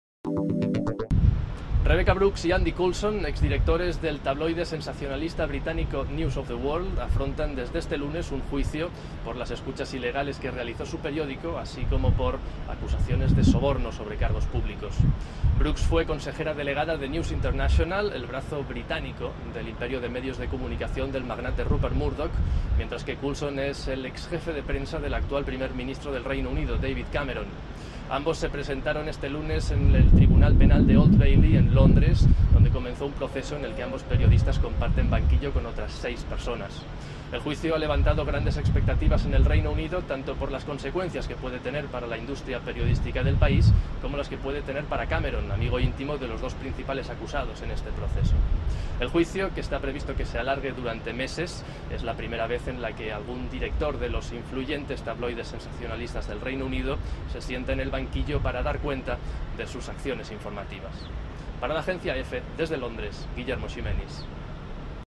Brooks y Coulson, en el banquillo por el caso de las escuchas en Reino Unido
Londres, 28 oct (EFE). (Imagen: Guillermo Ximenis).- Los exdirectores del "News of the World" Rebekah Brooks y Andy Coulson, dos de los periodistas más poderosos del Reino Unido, se sentaron hoy por primera vez en el banquillo de los acusados al inicio del histórico juicio por las escuchas ilegales practicadas en ese periódico.

Brooks y Coulson, ambos de 45 años, se personaron, junto a otros seis acusados, en la sala 12 del tribunal penal londinense de Old Bailey, que estaba abarrotado por docenas de abogados y al menos cincuenta periodistas repartidos en dos dependencias.

Durante la sesión de hoy, el tribunal se dedicó a seleccionar al jurado que asistirá a un proceso que, según indicó el juez, puede durar meses.

Rebekah Brooks, que fue consejera delegada de News International -antigua filial británica del imperio de Rupert Murdoch, ahora News UK- se sentó junto a su esposo, Charlie Brooks, también imputado, mientras que el resto de los procesados se situó a su derecha.

Se espera que mañana empiecen los alegatos de la fiscalía, entre restricciones impuestas a la prensa e incluso a los parlamentarios británicos, a quienes se ha pedido que se abstengan de hacer comentarios sobre el caso para evitar interferencias.

Este juicio suscita un gran interés en el Reino Unido, sobre todo en círculos políticos y mediáticos, porque por primera vez se revisa ante los tribunales la cultura informativa de los llamados tabloides o periódicos sensacionalistas, los más vendidos del país y de gran influencia.

Además, el proceso puede salpicar al Gobierno británico, pues tanto Brooks como Coulson son amigos personales del primer ministro, David Cameron, de quien el segundo fue jefe de prensa, y al magnate estadounidense Rupert Murdoch, propietario del ya desaparecido "News of the World", "The Sun" y "The Times" en el Reino Unido.

Además de Coulson y el matrimonio Brooks, están acusados el exjefe de noticias del "News of the World" Ian Edmonson, de 44 años; el antiguo director gerente Stuart Kuttner, de 73, y Cheryl Carter, exayudante personal de Rebekah Brooks, de 49 años.

También fueron procesados el exjefe de seguridad de News International Mark Hanna y el antiguo corresponsal de realeza Clive Goodman, de 56 años, quien ya fue condenado por las escuchas cuando el caso salió primeramente a la luz pública en 2006.

Brooks y Coulson afrontan cargos de interceptación de comunicaciones -concretamente, acceder ilegalmente a buzones de voz de móviles- entre 2000 y 2006 con Edmonson y Kuttner, así como de posibles sobornos a cargos públicos, de lo que también está acusado Goodman.

Asimismo, Rebekah Brooks está acusada de obstrucción a la justicia en 2011 (cuando estalló el caso) con Carter, por haber intentado eliminar archivos de "News of the World", y con su esposo y Hanna por intentar ocultar a la Policía documentos y ordenadores relevantes al caso.

Los ocho imputados, que están en libertad bajo fianza, niegan los cargos.